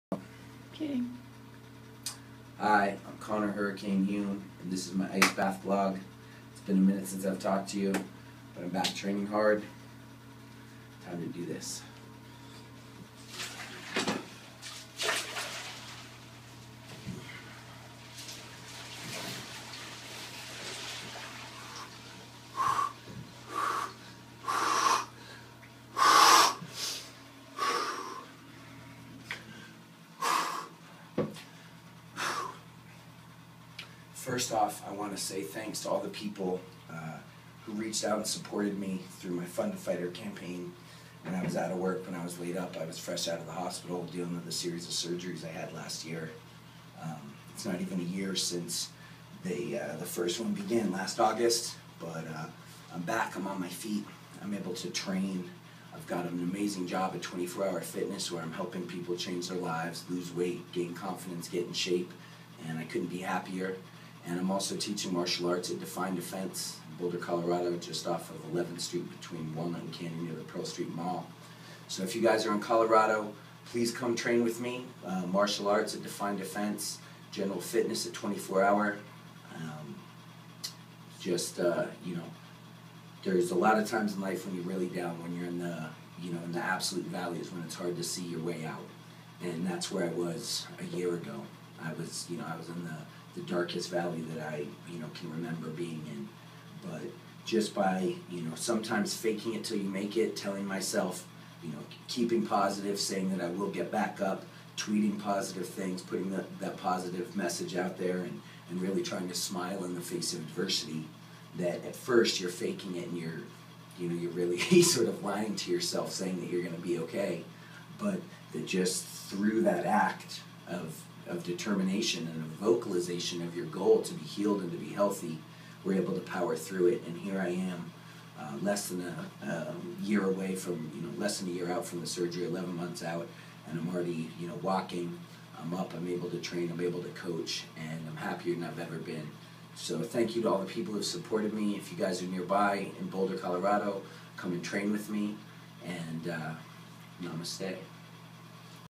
Ice Bath Blog: Fake it till you make it.
Talking about setting positive intention through vocalization.